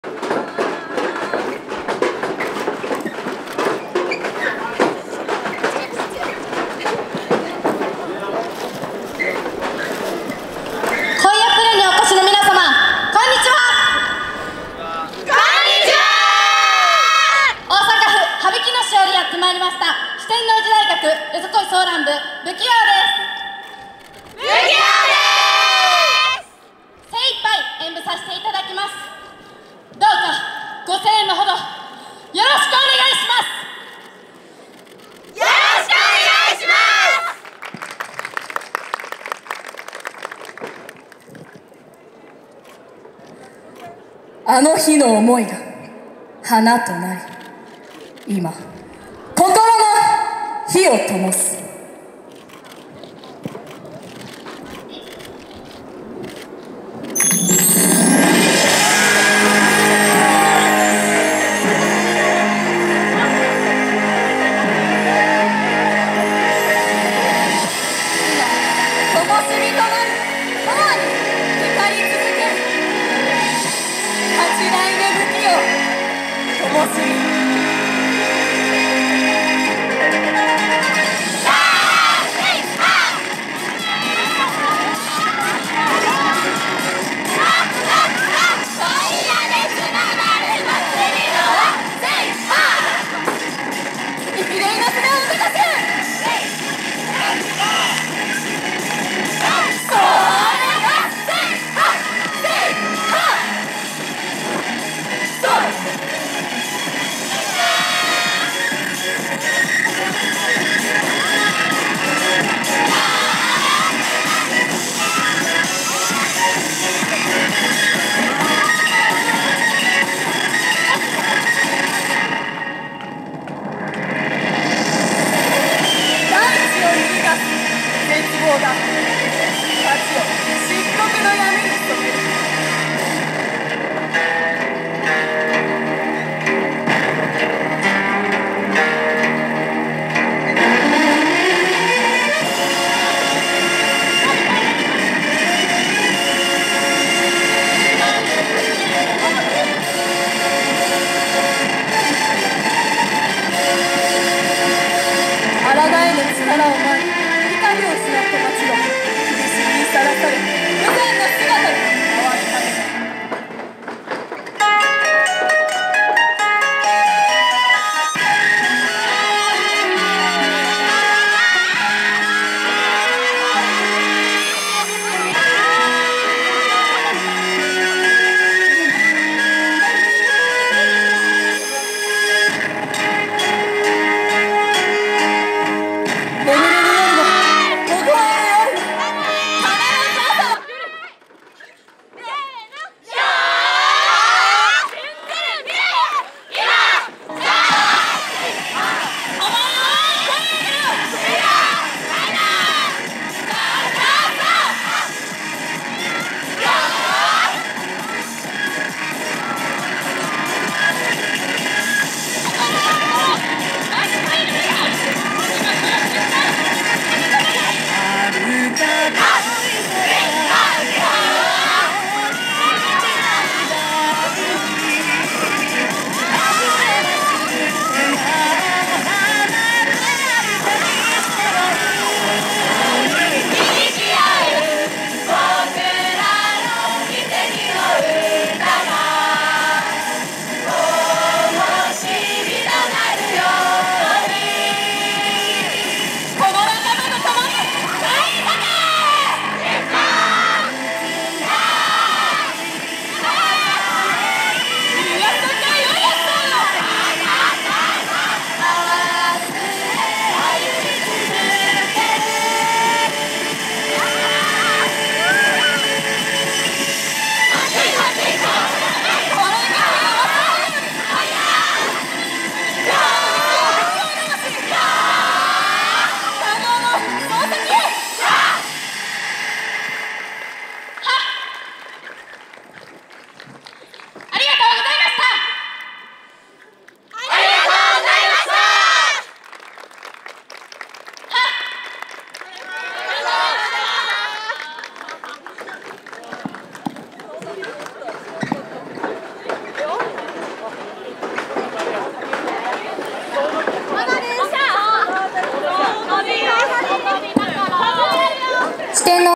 [4K] 四天王寺大学 YOSAKOIソーラン部仏喜踊 こいやプレ 2018 (丼舞Re:Festival) UMEDAIガーデン (中央)
こいやプレ 2018
こいやプレゼンツ 丼舞Re: Festival
2018年5月20日(日)
UMEDAIガーデン